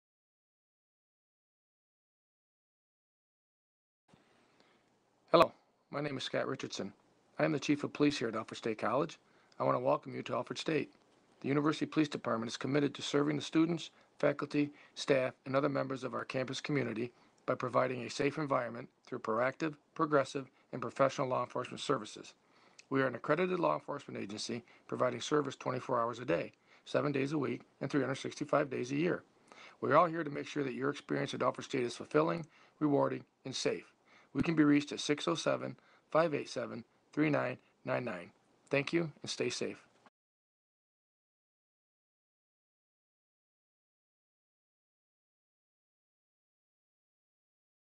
Student Services University Police
1-800-4-ALFRED (425-3733)

Law enforcement is coordinated on the Alfred State campus by the University Police Department, a fully accredited law enforcement agency. This department consists of a chief of police, department secretary, four lieutenants, and eight sworn police officers. All officers in this department are certified police officers in New York State.

The University Police department also has four full time dispatchers who manage the communications center and are responsible for receiving campus emergency calls as well as routine calls for service. The blue emergency call boxes across campus are also answered by the UPD dispatchers. These emergency blue light telephones are communication devices that place a person in direct contact with University Police by the simple touch of a button. Once pressed, a speaker phone allows the dispatcher to talk with the caller and send help immediately. Emergency blue light telephones are only to be used in emergency situations.

The University Police Department at Alfred State conducts vehicular, bicycle, UTV and foot patrols 24-hours a day, 365 days a year on campus and in the residence hall areas.